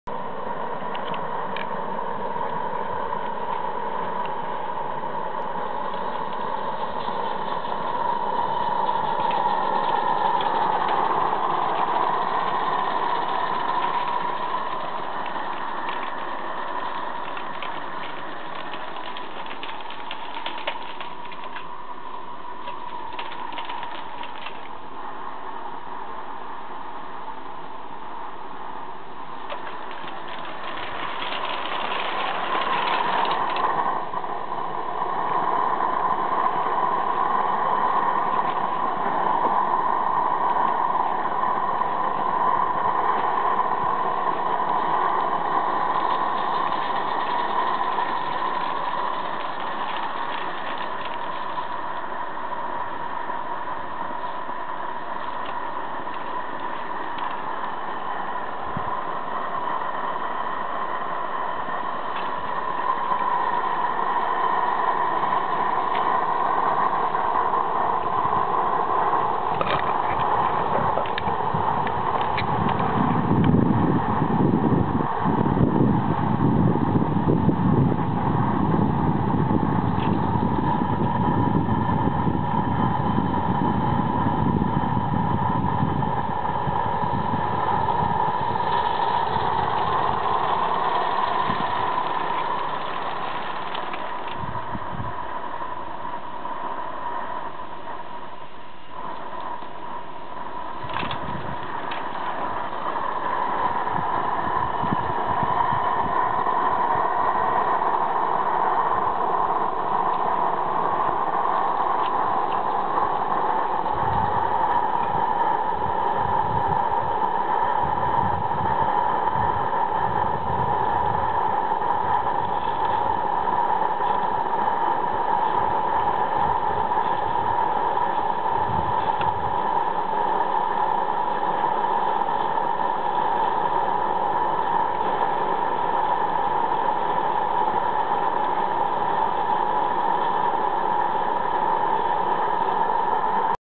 IK Locomotive built by Accucraft DE and Saxonian Train
The derailment of the first axis of the first wagon is due to a defective coupling hook